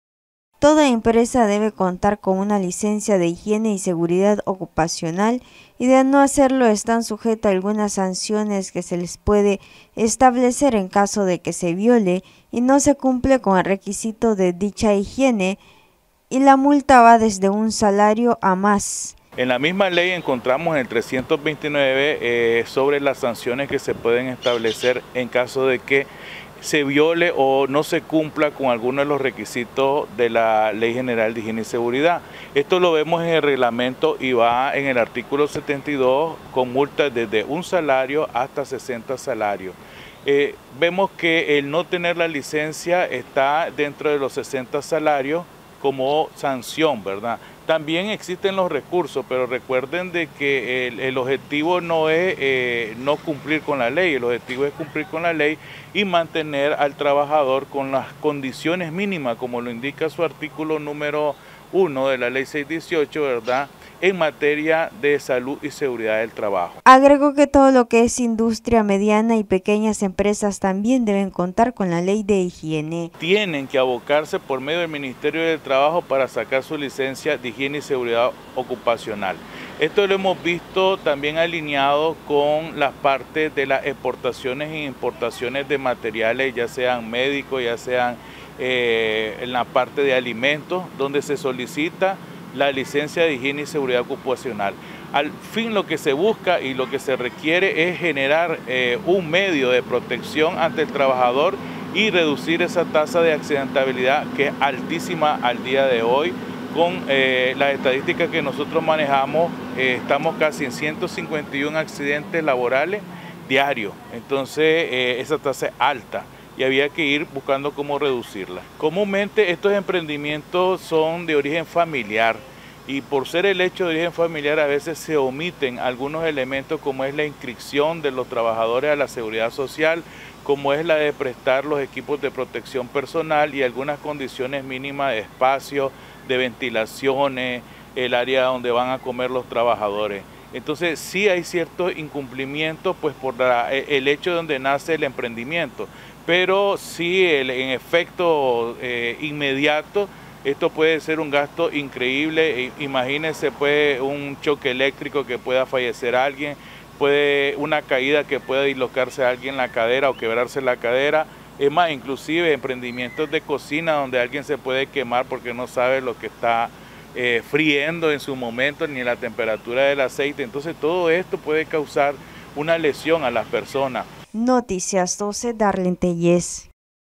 Noticias12Nicaragua ¿Por que toda empresa debe contar con su licencia de higiene y seguridad?
¿Por que toda empresa debe contar con su licencia de higiene y seguridad?